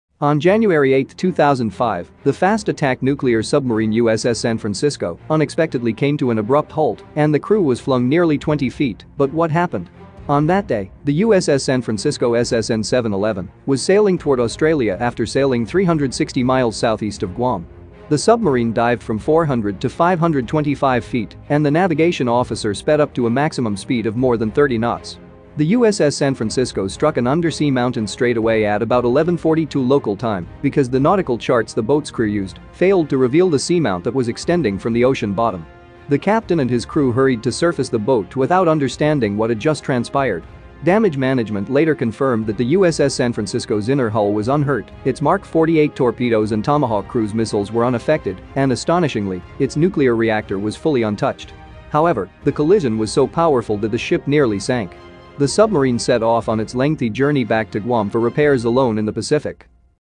When a US Submarine Collided With An Undersea Mountain In 2005
On January 8, 2005, the fast-attack nuclear submarine USS San Francisco unexpectedly came to an abrupt halt, and the crew was flung nearly 20 feet, but what happened? On that day, the USS San Francisco SSN-711, was sailing toward Australia after sailing 360 miles southeast of Guam. The submarine dived from 400 to 525 feet, and the navigation officer sped up to a maximum speed of more than 30 knots. The USS San Francisco struck an undersea mountain straight away at about 11:42 local time, because the nautical charts the boat's crew used, failed to reveal the seamount that was extending from the ocean bottom. The captain and his crew hurried to surface the boat without understanding what had just transpired. Damage management later confirmed that the USS San Francisco's inner hull was unhurt, its Mark 48 torpedoes and Tomahawk cruise missiles were unaffected, and astonishingly, its nuclear reactor was fully untouched. However, the collision was so powerful that the ship nearly sank. The submarine set off on its lengthy journey back to Guam for repairs, alone in the Pacific.